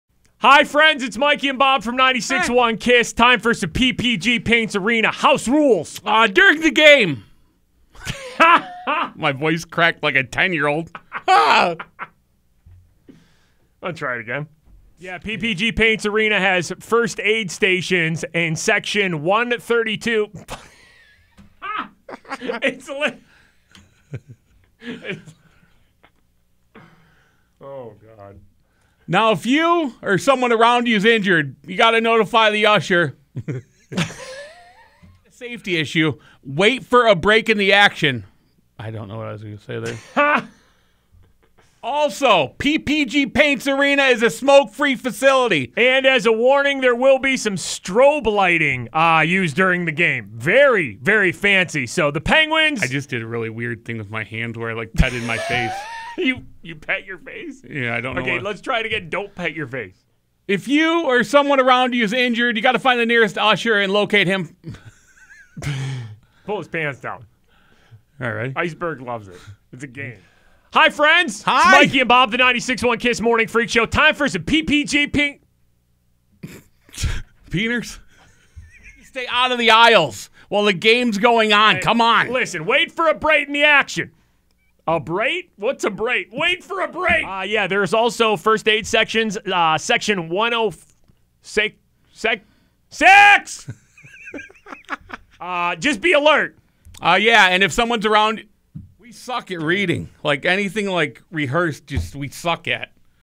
Screwing up reading PPG Paints Arena house rules
The Penguins asked us to read the PPG Paints Arena house rules and. we're not very good at these things